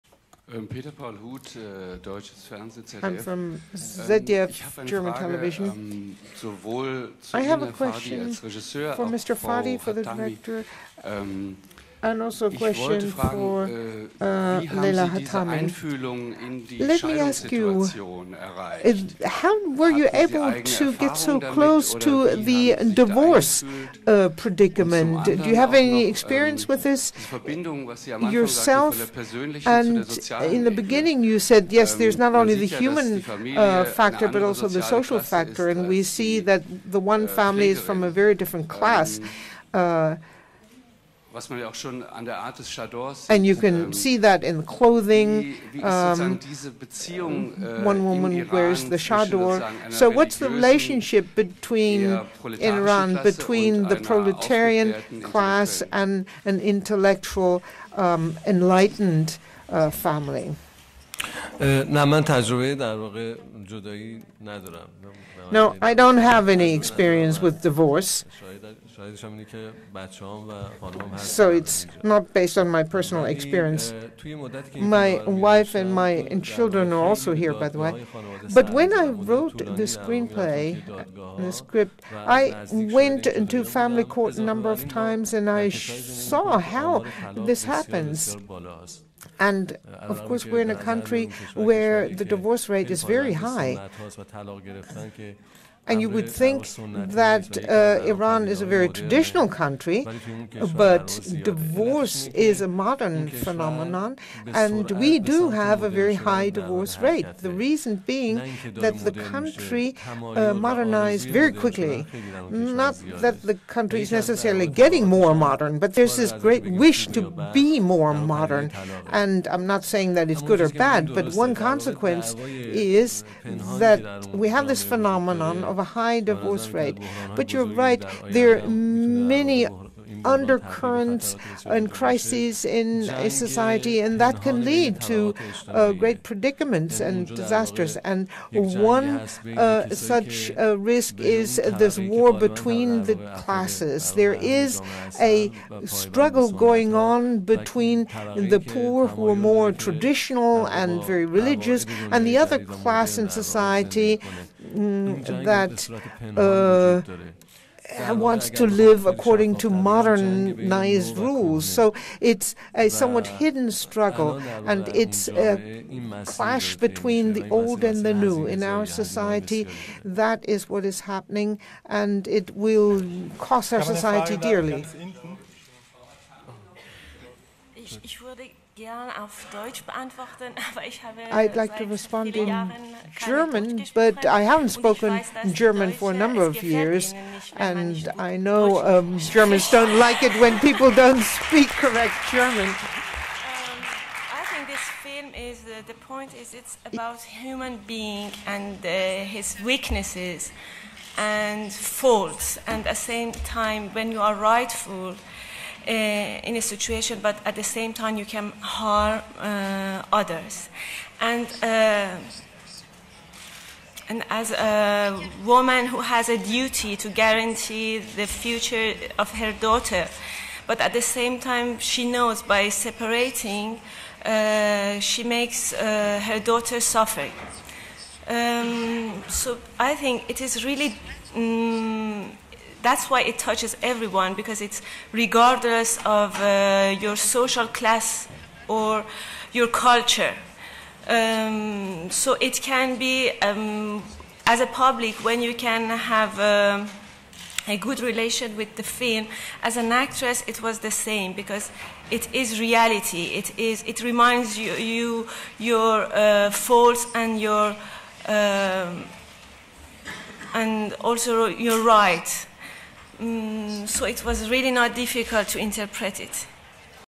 Leila Hatami speaking German - Conference Press - Berlin Film Festival 2011
Jodaeiye Nader az Simin, Golden Bear Winner for the Best Film, Berlin Film Festival 2011